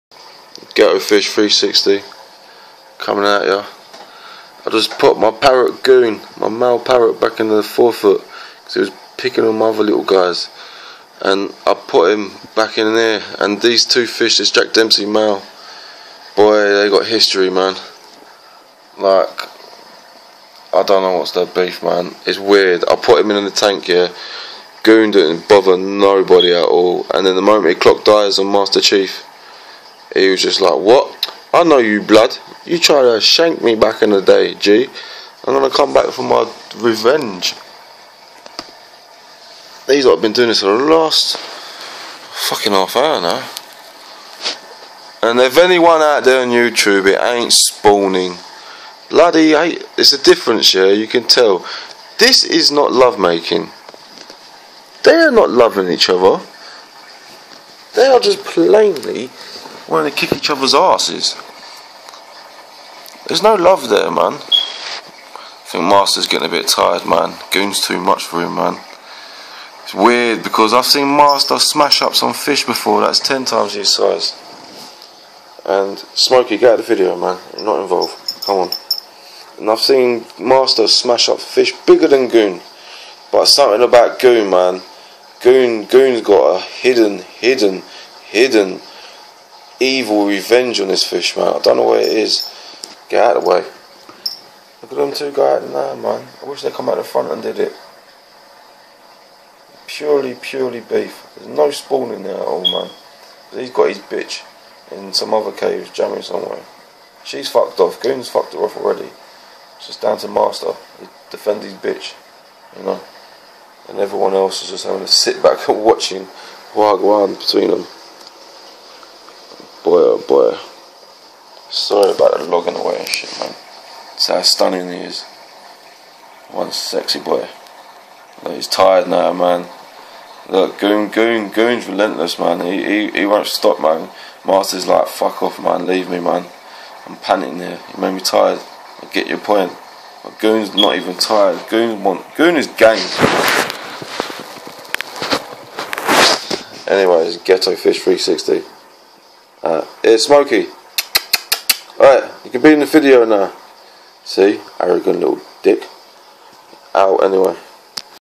Jack Dempsey vs parrot fish Ghettofish360
Male jack Dempsey master chief  vs Male patriot fish goon
These two hate each got to keep changing the tank lay out
Every week as they always seem to beef lol